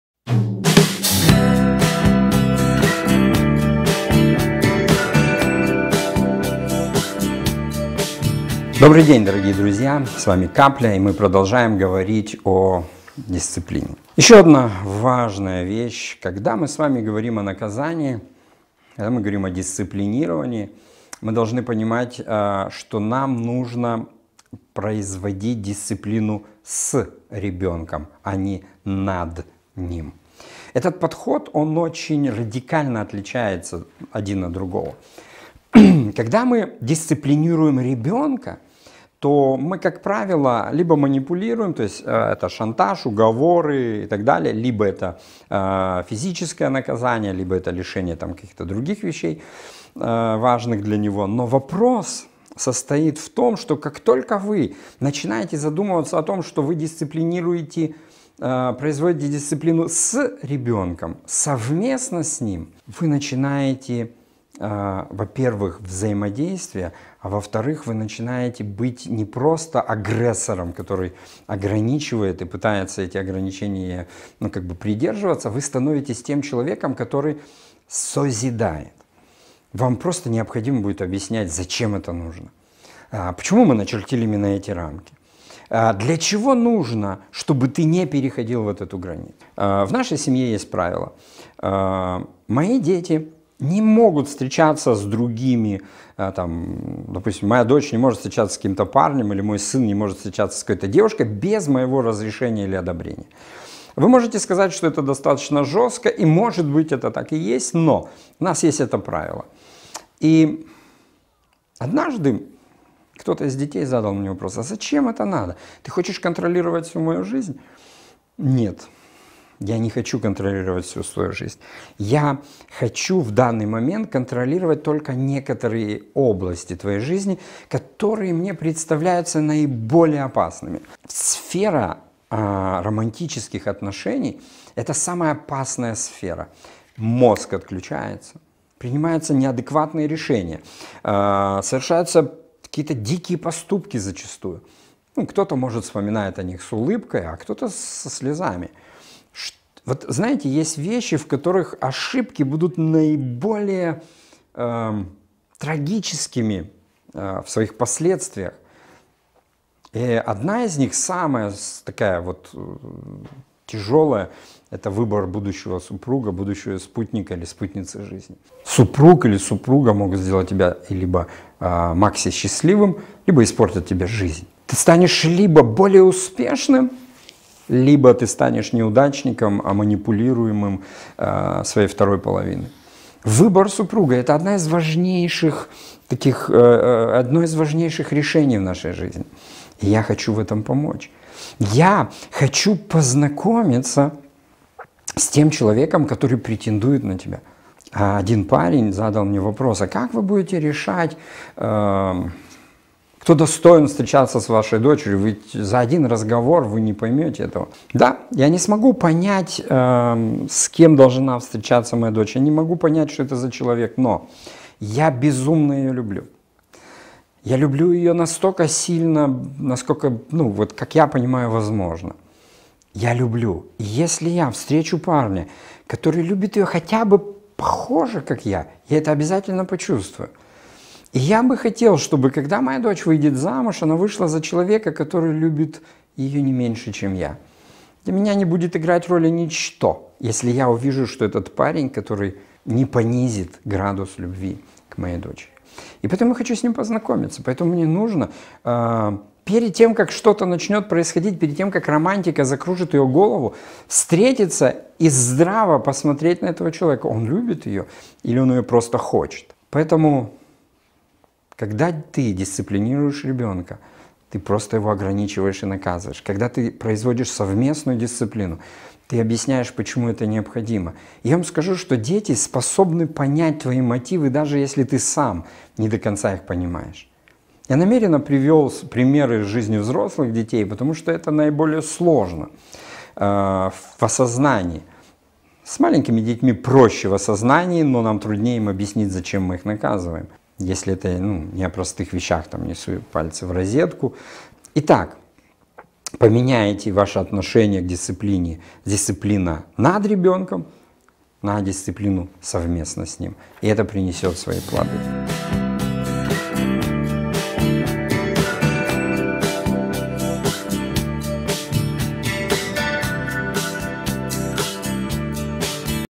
ДИСЦИПЛИНИРОВАНИЕ "С" РЕБЕНКОМ
Программа «Капля». Говорим об Отцовстве, отвественности, воспитании и дисциплинировании ребенка.  Воспитание детей. Изменить подход воспитания.